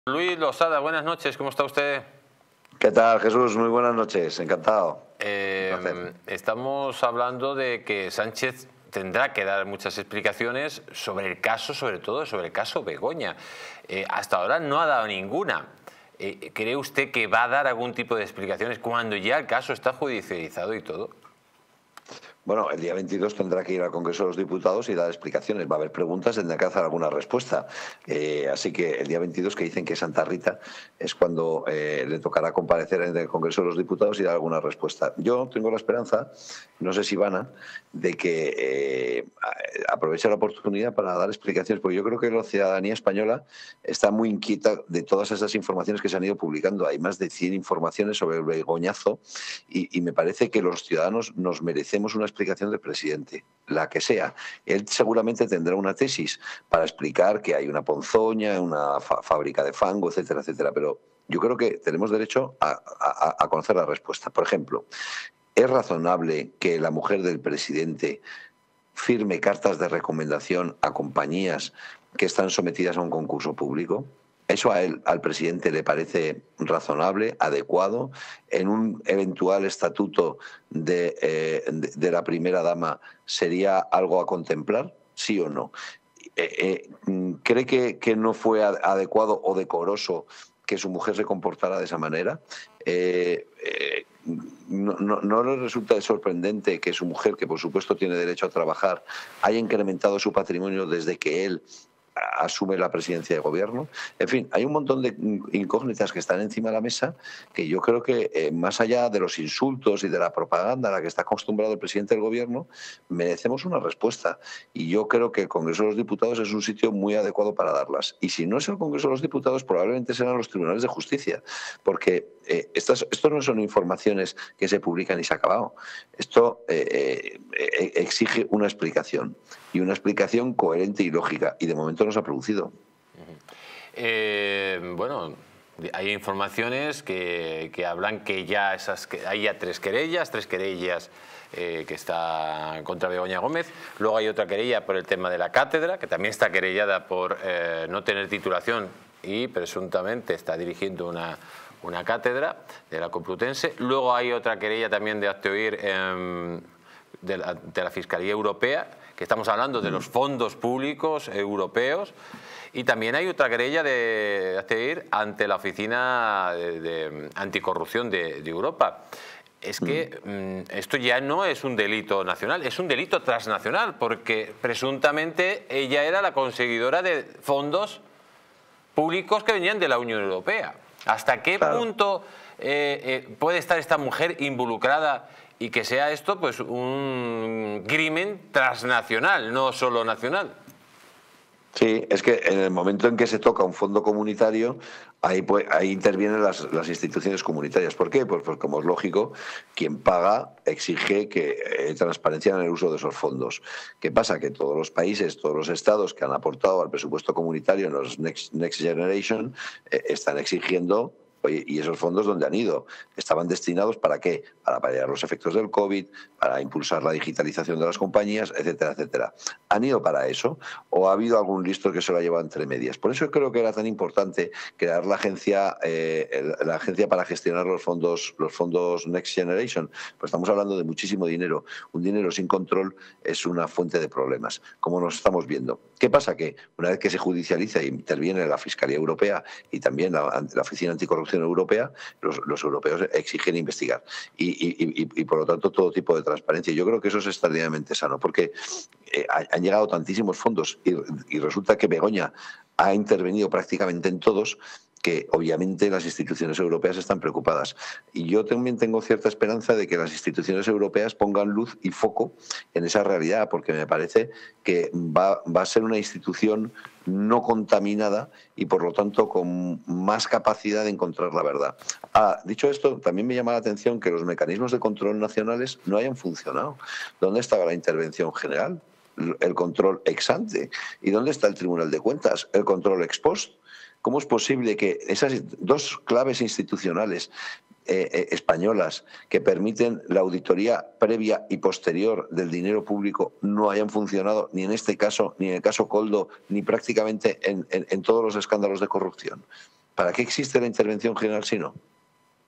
🔴EL CIRCO DE SÁNCHEZ ESTE MIÉRCOLES EN EL CONGRESO PARA TAPAR LOS NEGOCIOS DE BEGOÑA🔴
¡No te pierdas el circo de Sánchez este miércoles en el congreso para tapar los negocios de Begoña! ¡Sintoniza Distrito TV para más detalles!🔴
🔴Conviértete en miembro de este canal para disfrutar de ventajas: